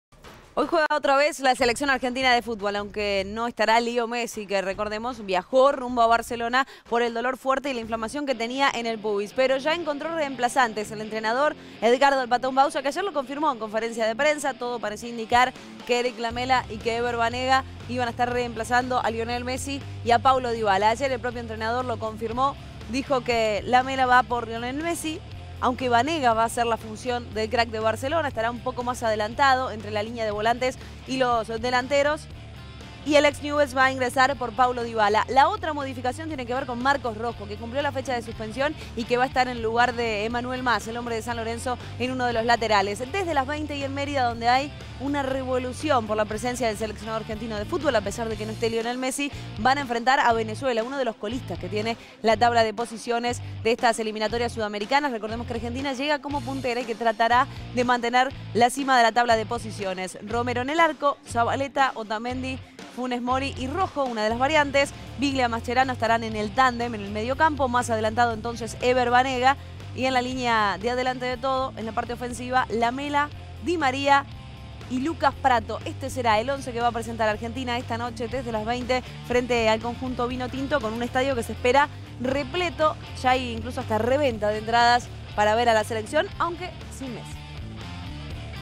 TV Pública Noticias - La Selección, sin Messi, juega en Venezuela
Los dirigidos por Edgardo Bauza enfrentarán a las 20 en Mérida a la "Vinotinto", que marcha última en la tabla de posiciones de las eliminatorias sudamericanas. El técnico confirmó que ensayará varios cambios respecto del equipo que venció a Uruguay por 1 a 0 en Mendoza y alcanzó la punta. En consecuencia, la Selección formará con Sergio Romero en el arco; Pablo Zabaleta, Nicolás Otamendi, Ramiro Funes Mori y Marcos Rojo en la defensa; Lucas Biglia y Javier Mascherano en la contención del mediocampo; Erik Lamela, Ever Banega y Ángel Di María en la creación; y Lucas Pratto como delantero neto. Emitido por la Televisión Pública Noticias, el martes 6 de septiembre de 2016.